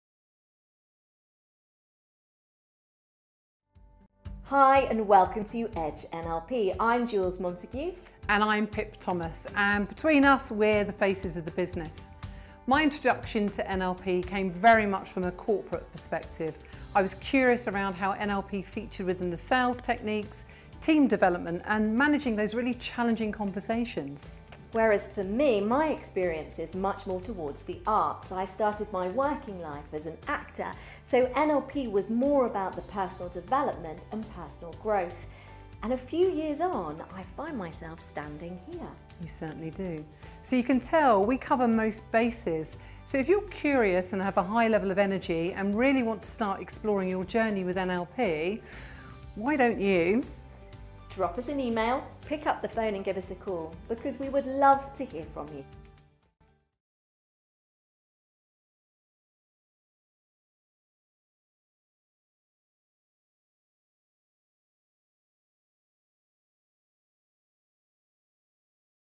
Edge NLP - An Introduction by Pip and Jools
Pip Thomas and Jools Montague introduce Edge NLP and talk about the exciting NLP training services they offer ...